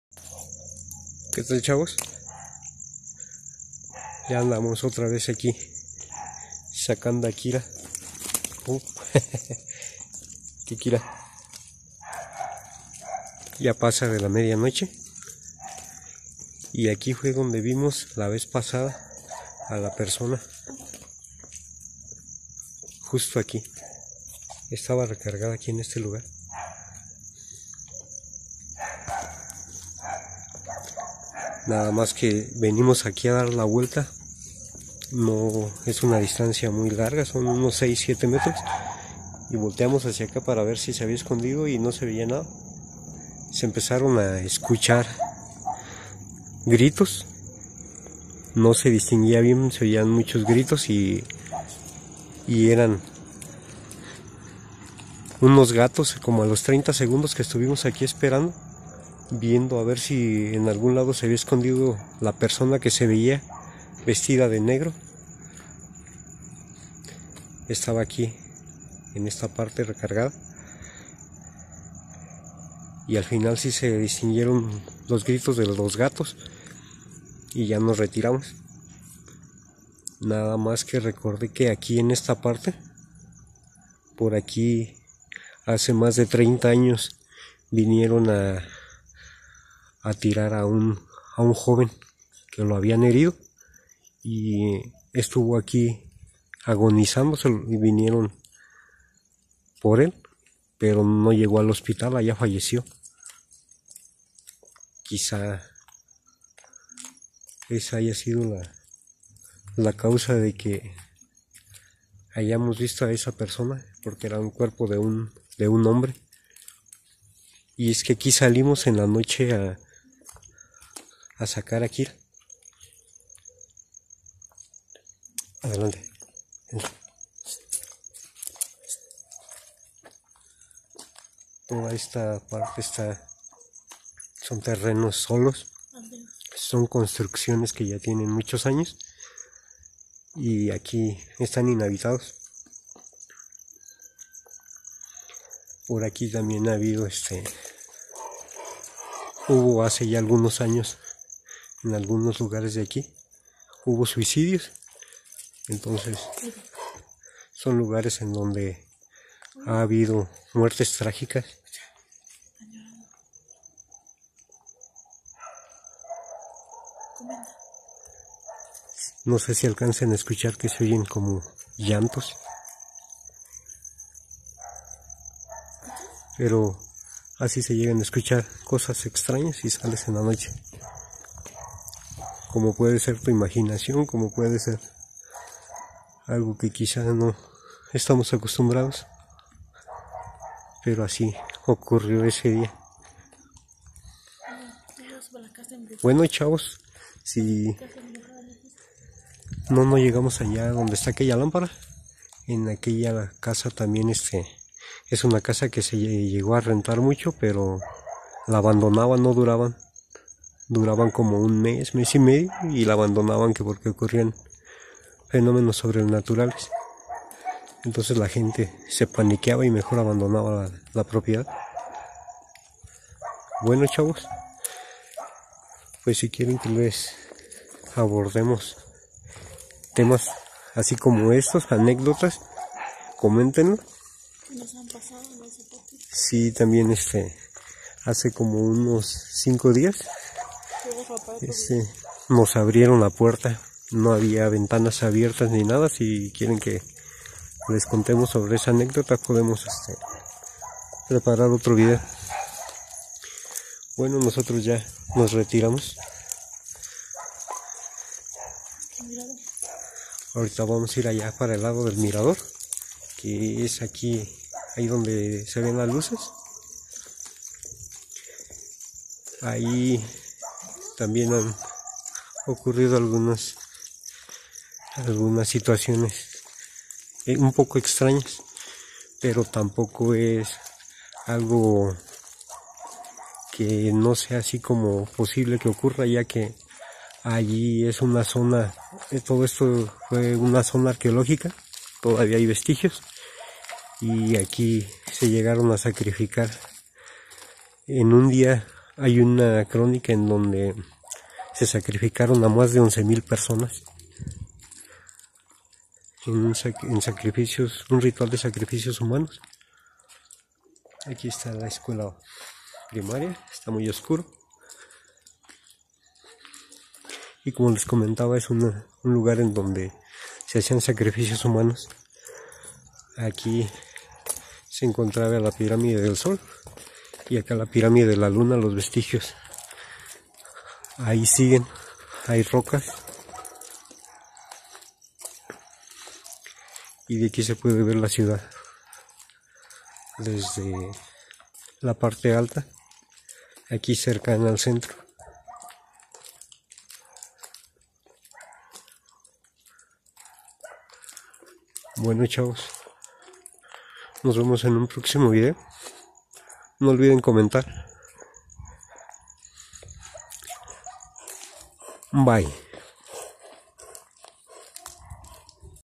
FANTASMAS EN LA MADRUGADA [SEGUNDA PARTE] - SE ESCUCHAN LLANTOS DURANTE LA GRABACIÓN.
Continuamos recorriendo algunos lugares después de la media noche, pasamos por donde ocurrieron algunas muertes trágicas...